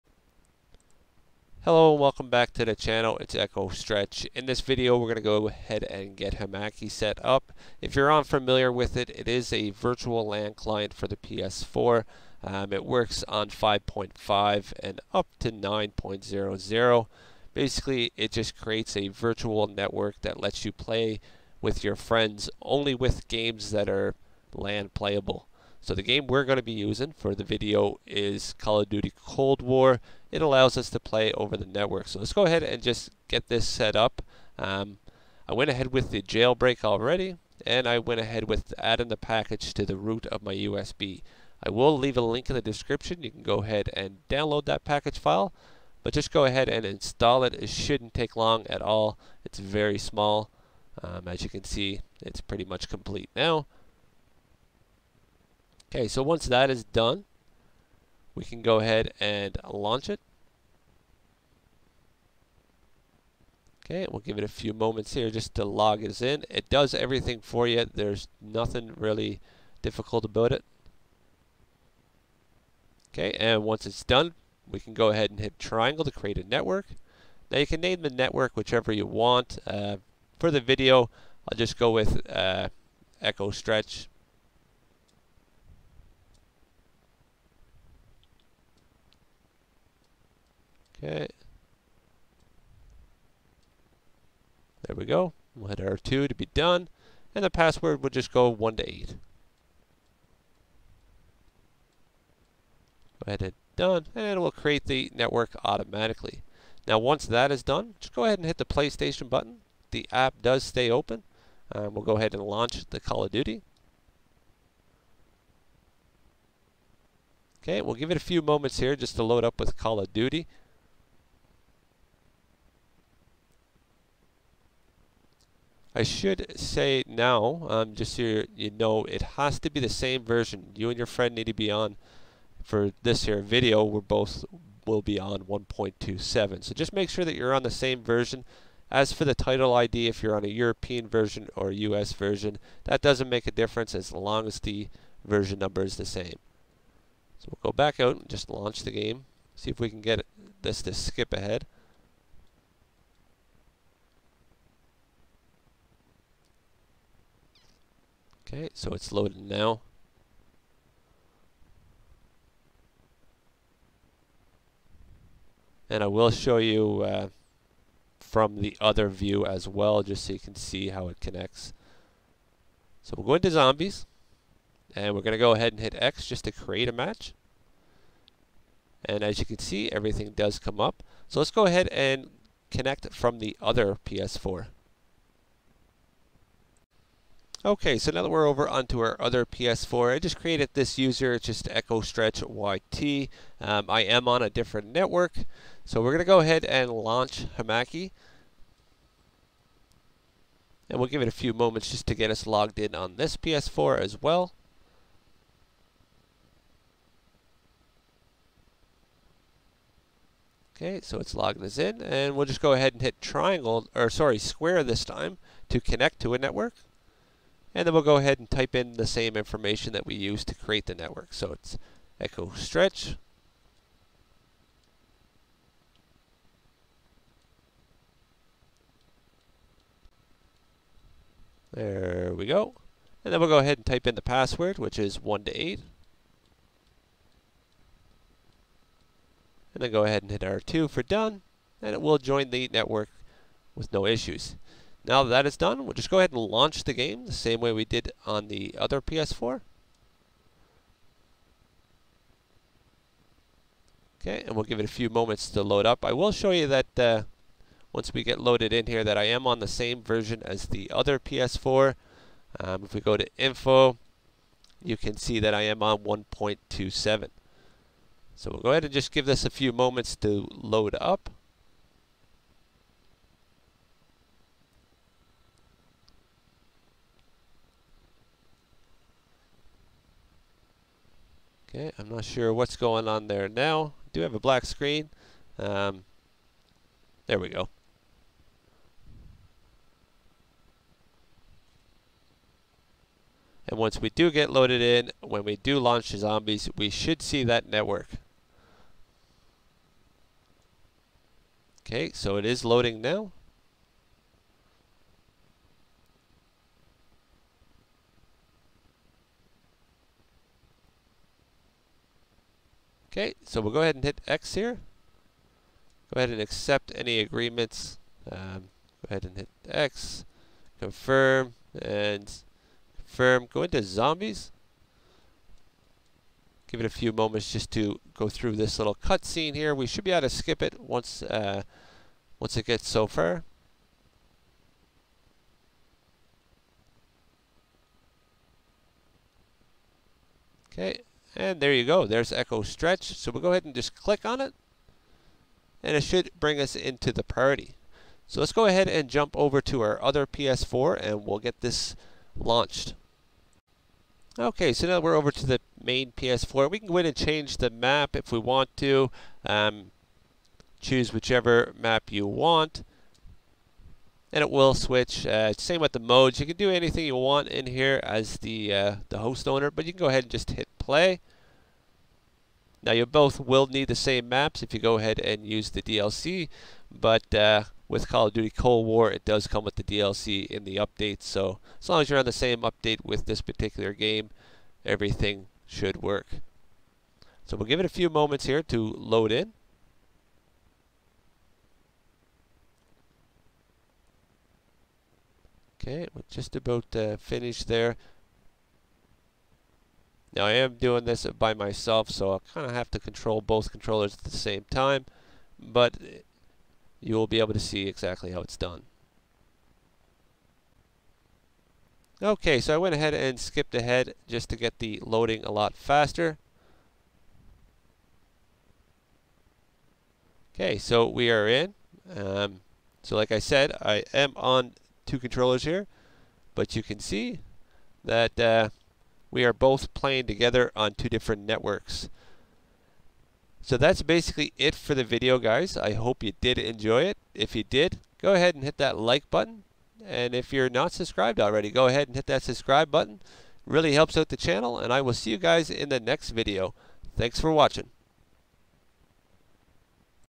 How To Play Lan Games Online On A Jailbroken PS4 Over LAN With Hamachi
Download

➭Activate PSN Accounts Offline To Enable Remote Play & Copy Save Files From USB 9.00 And Lower

➭How To Use PSN Offline Web Activator To Activate Your PS4 Account on the 9.00 Jailbreak